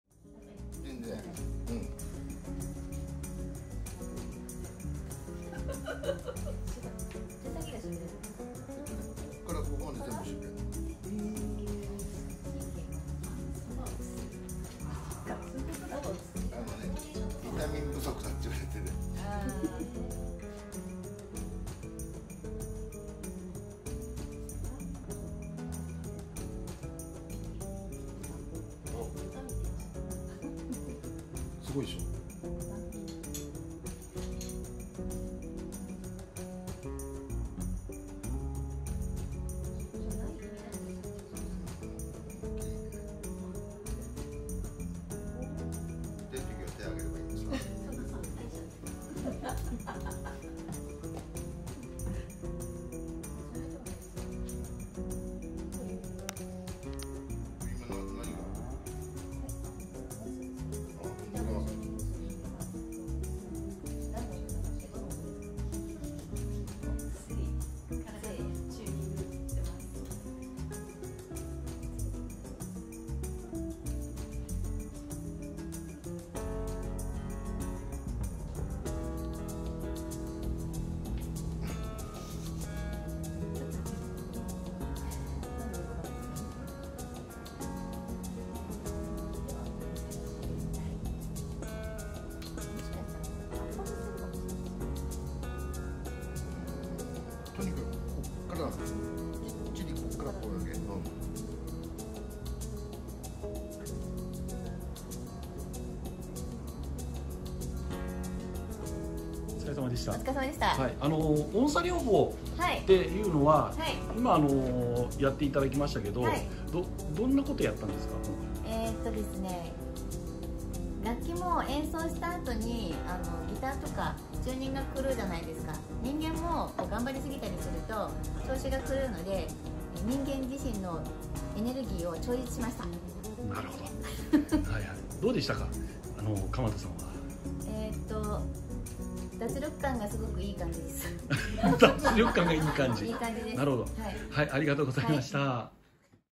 Hitomi Ory の音叉療法やってみた！！
Hitomi OryさんがGOEMON BAND のギターリスト Kさんに施術をしてみた。

代替療法ホームサロン Touch The Haertstrings
Hitomi Oryまで♪